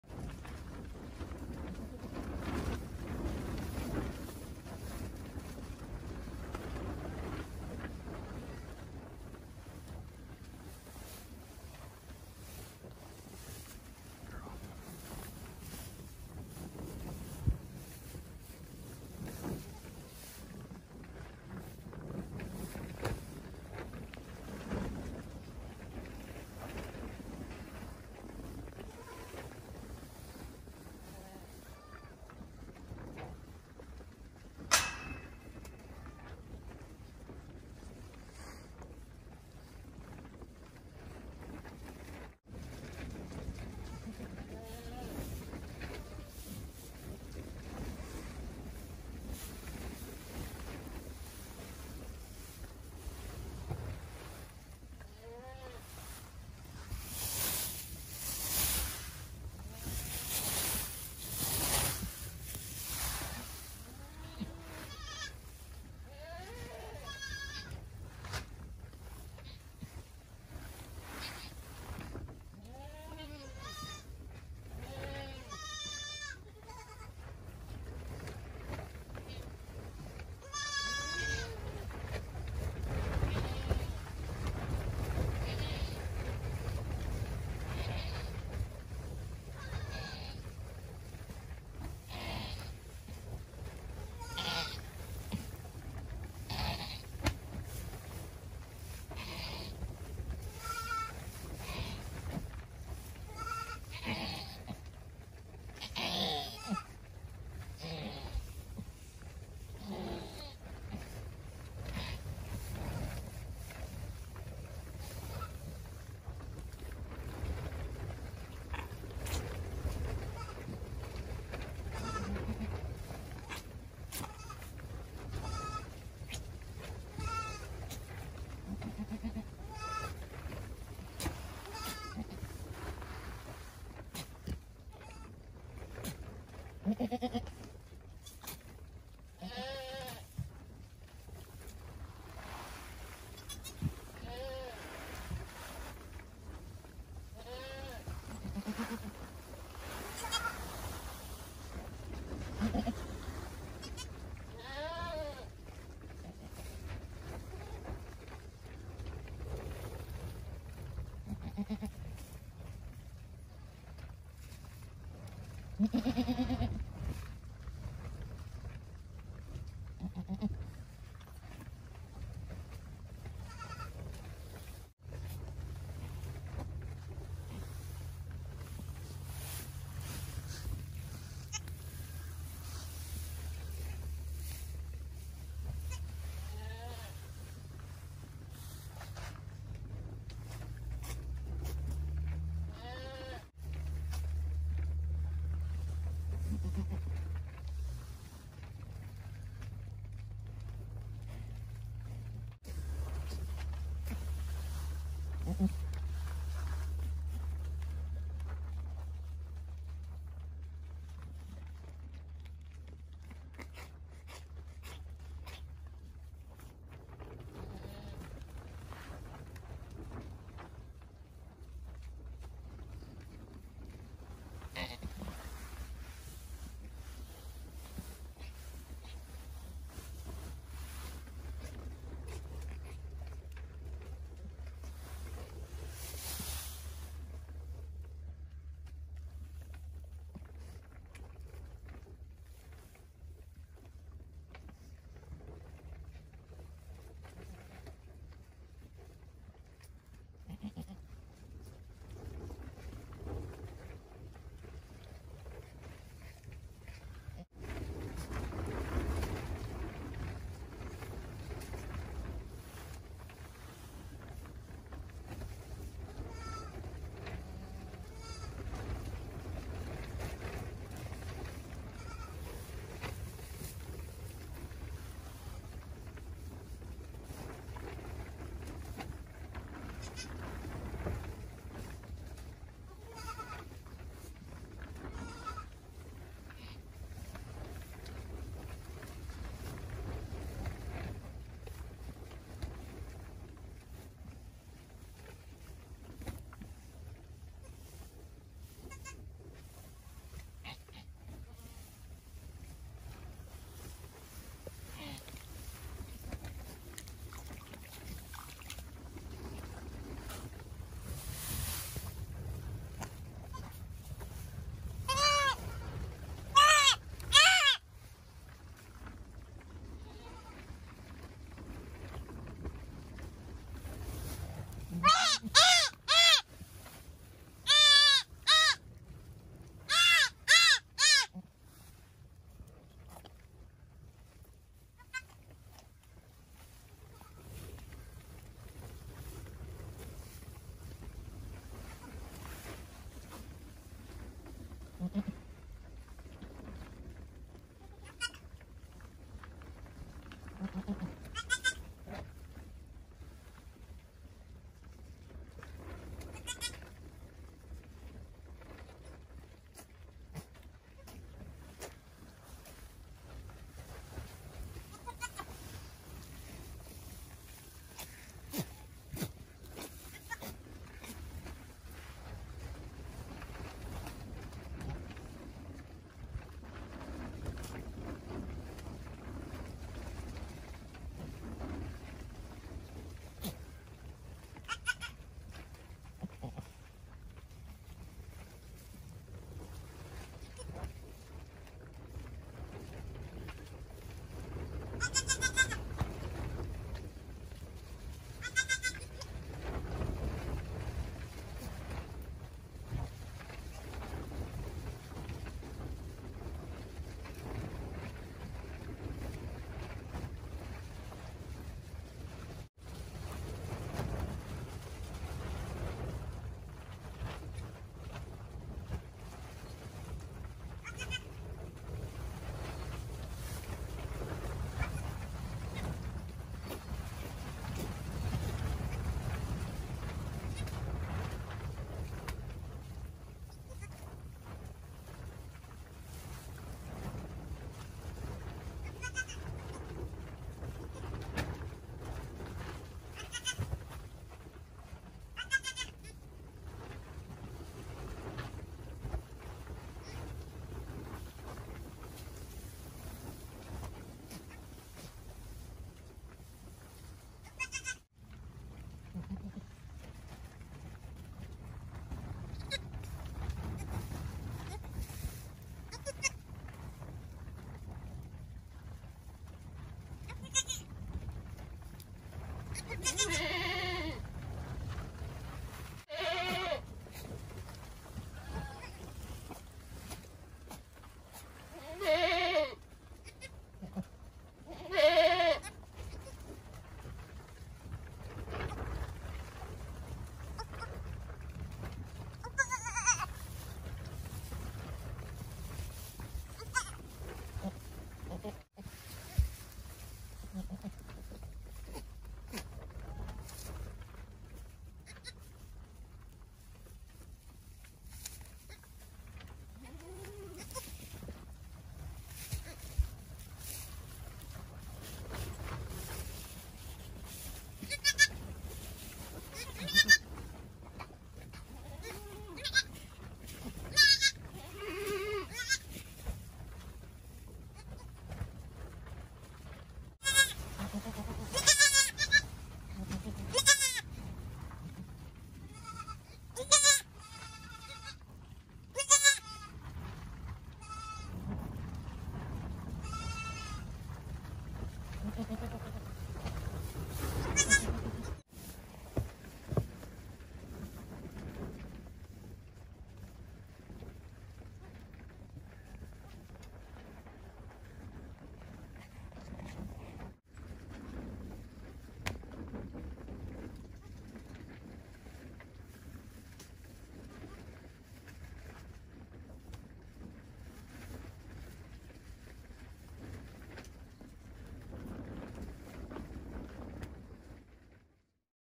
My GOAT, Jackie, gave birth to twins.
My goat gave birth to twins!! Jackie, who is actually my daughter’s Grand Champion goat, had twin babies.  It was awesome to catch the whole thing on video.  You get to see the entire delivery of both babies, a boy and a girl. Watch and see Jackie become a mother for the first time, the babies first steps, and nursing for their first time.